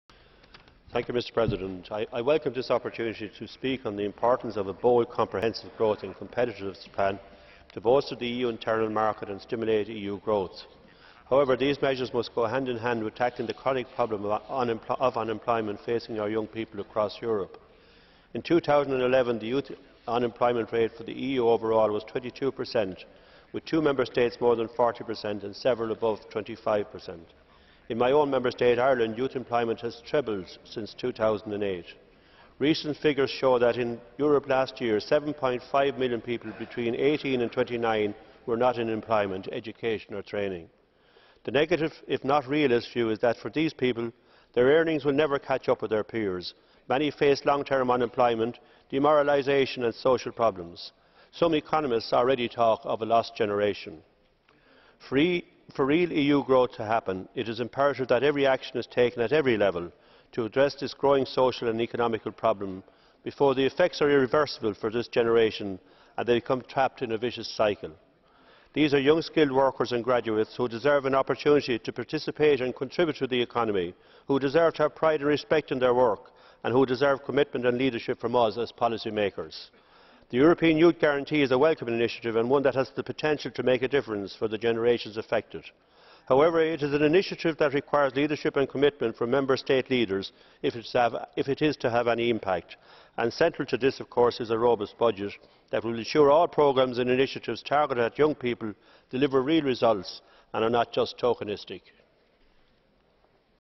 Liam Aylward on EU growth
Speech by Liam Aylward [MEP / Alliance of Liberals and Democrats for Europe / European Parliament] on :
EU growth

Aylward-OR_121213_113427_mesp_en